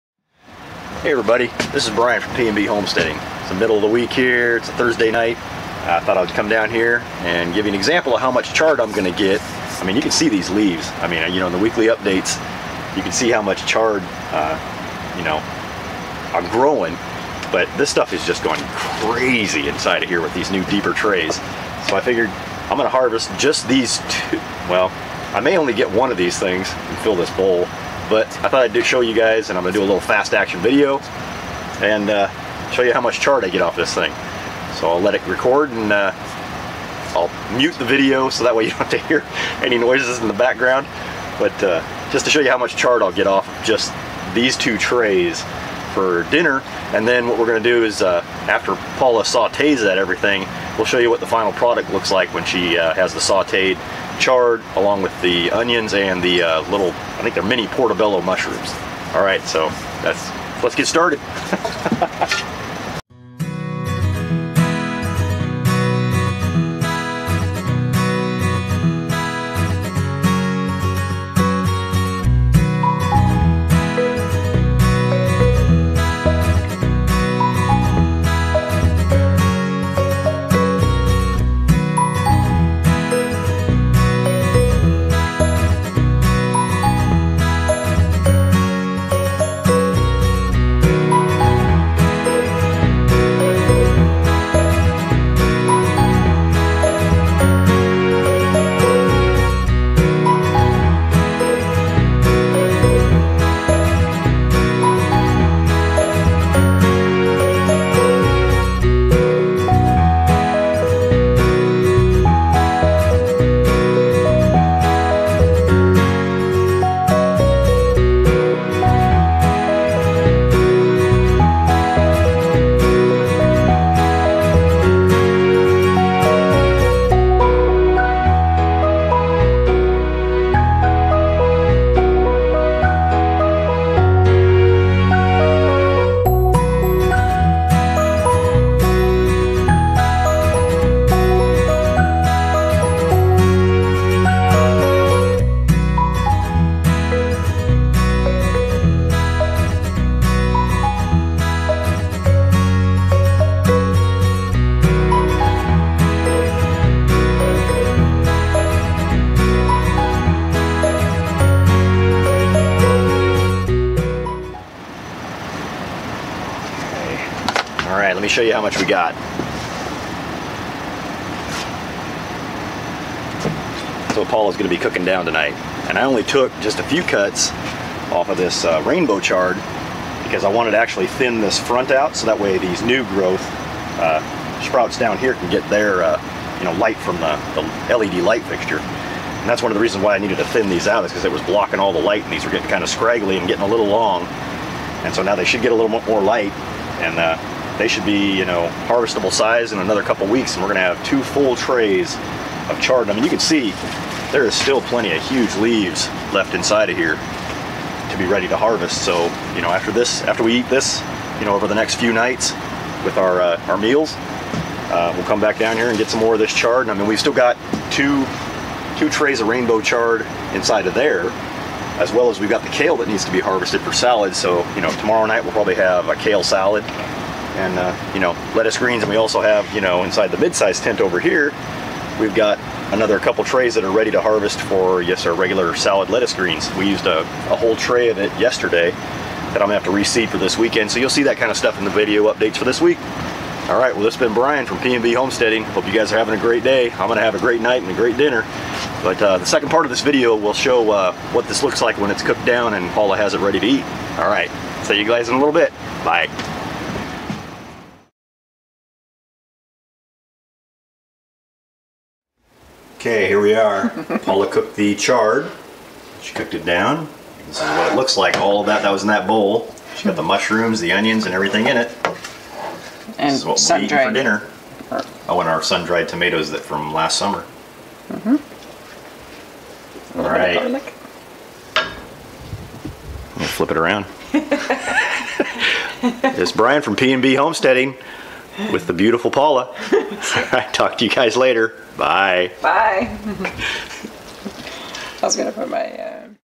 Harvesting swiss chard and sautéing for dinner
Hi Everyone, here's an extra video before the regular weekend updates. I'm harvesting our chard from the salad bar led grow bench and then Paula is sauteing it for our evening meal...yum. Enjoy the video! :-)